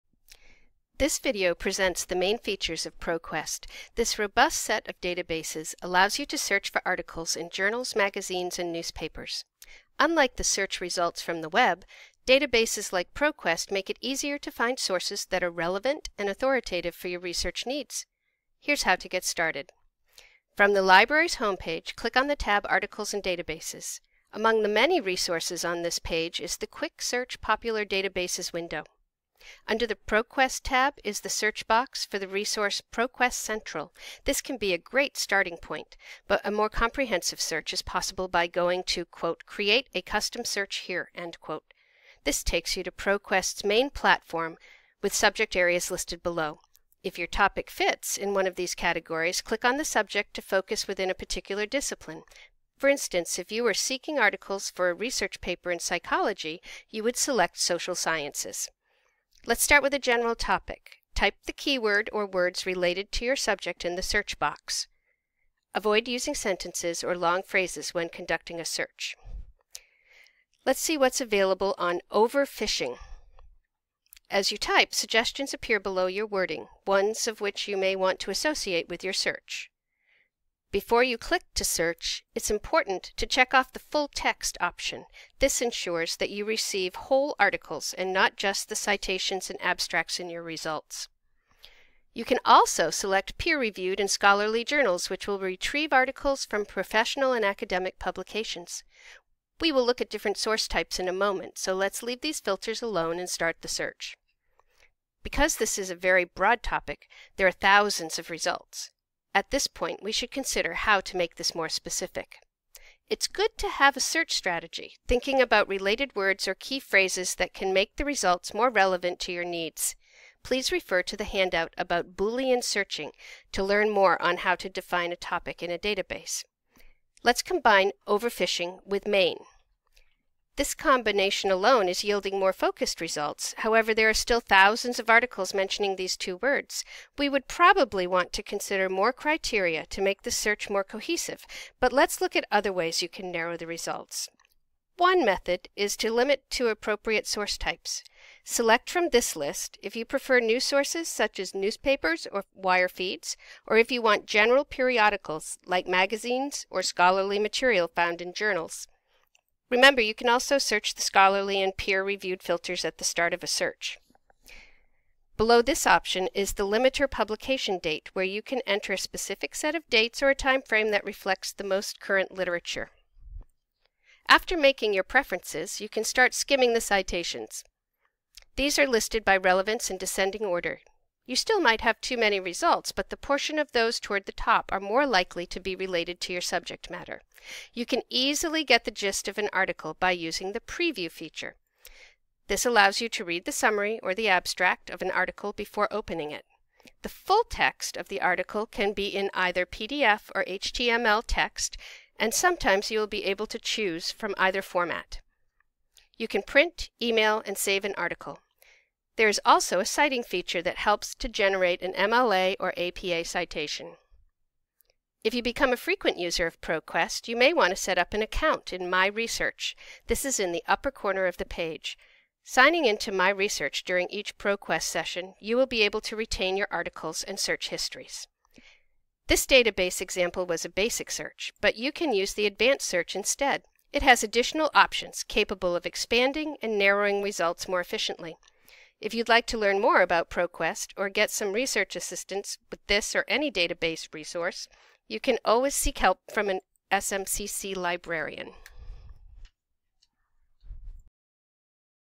Searching ProQuest
This video focuses on accessing ProQuest and performing basic search and filtration functions. Similar info about EBSCO is in a separate video.